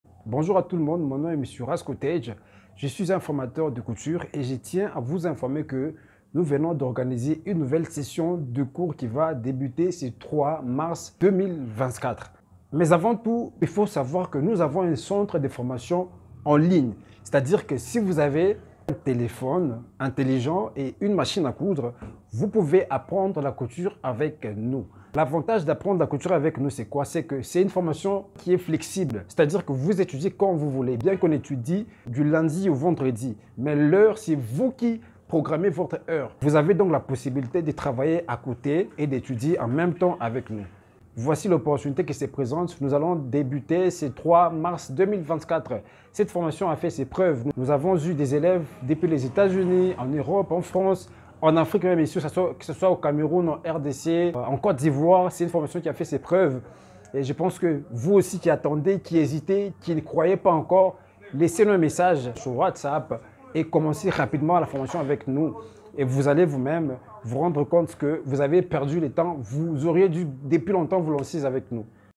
Nouvelle Formation de Couture en Ligne le 04 Mars 2024 | FASHION ACADEMY CENTER 👔👖🌍
Nouvelle session à partir 04 mars 2024.

Faites vous enregistrer pour ne pas louper cette opportunité d'apprendre la couture depuis la maison. 👔👖👕